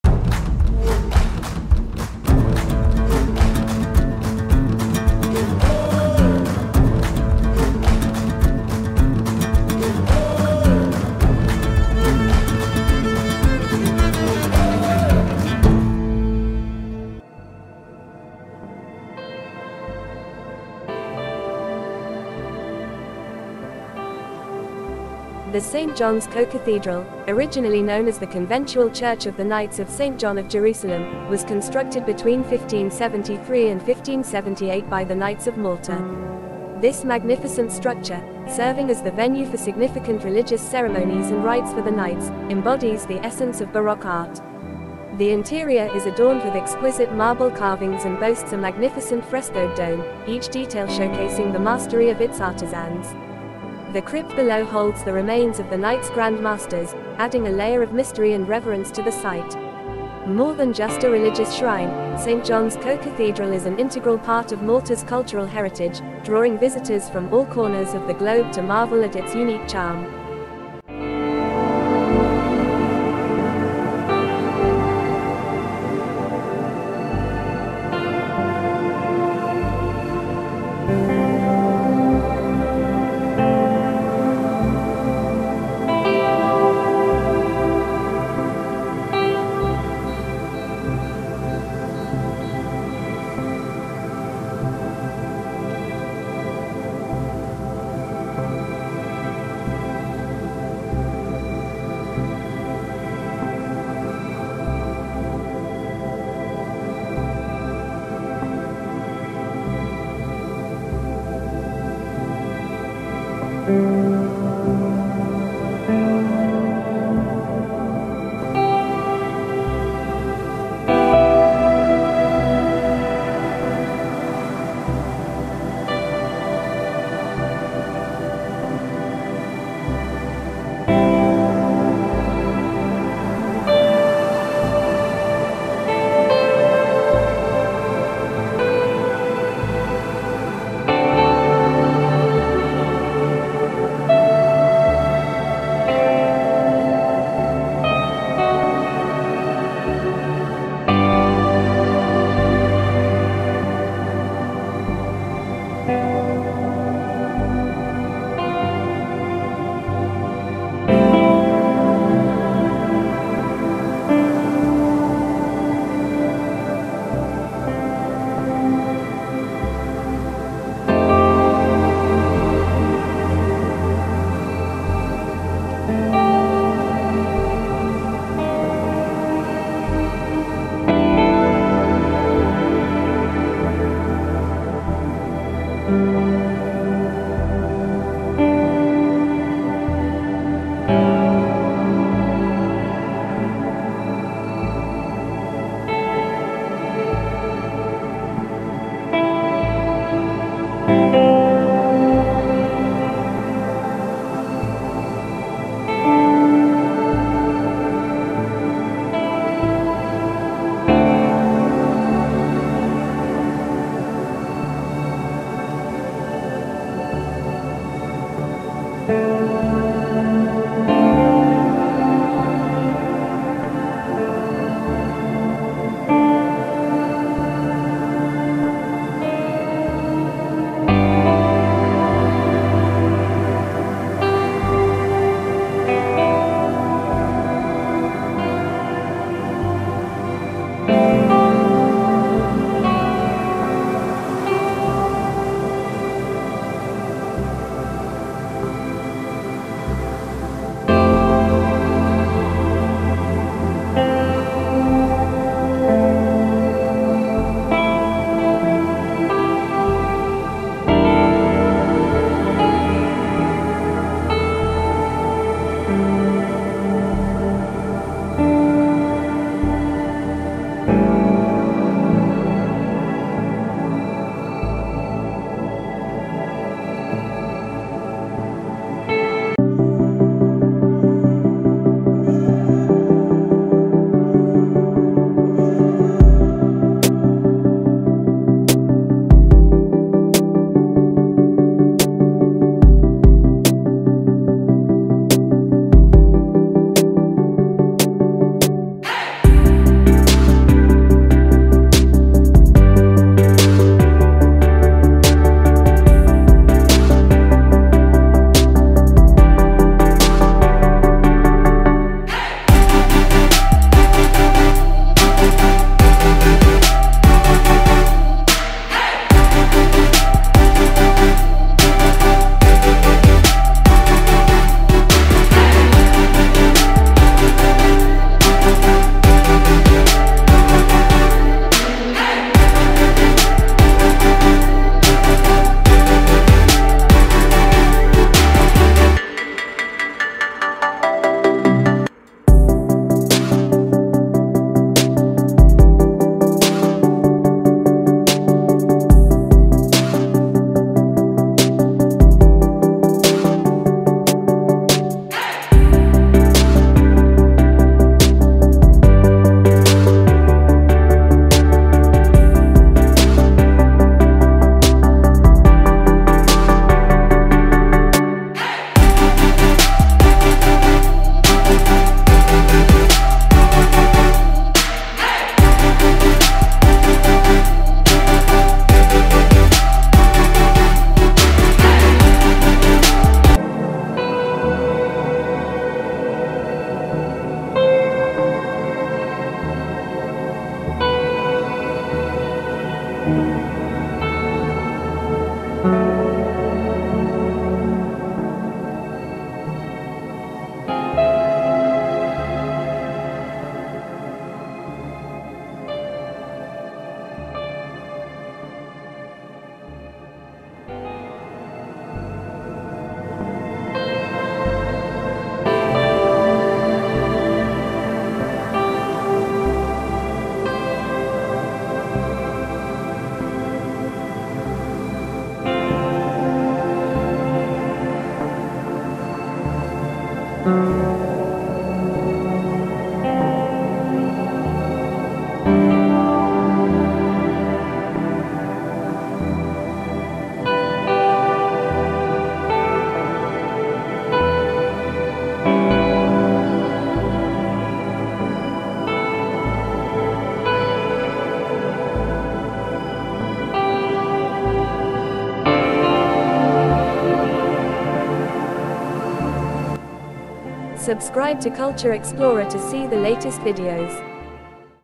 Stunning! St. John's Co-Cathedral in Malta | Culture Explorer
Visiting St. John's Co-Cathedral in Malta.

Find out more at culture-explorer.co.uk